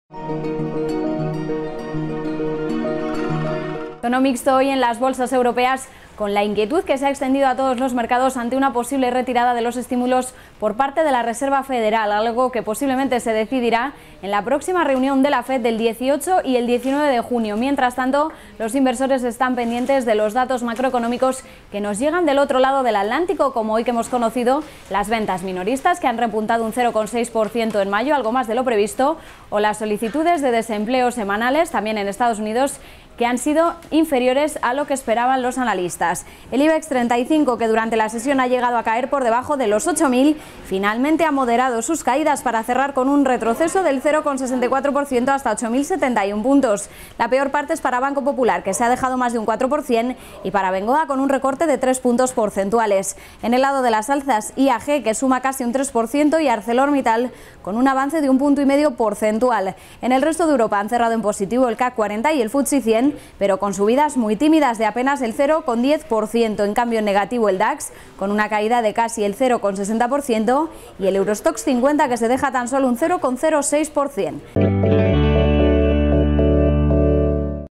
El Ibex vuelve a las caídas por el miedo a que la FED corte el grifo (13/06/2013)
Hoy el Ibex35 ha moderado sus caídas, aunque de nuevo a vuelto a cerrar en negativo. Con la retirada de estímulos en el horizonte, los mercados europeos no muestran un signo claro. Mientras tanto, los protagonistas son los datos macroeconómicos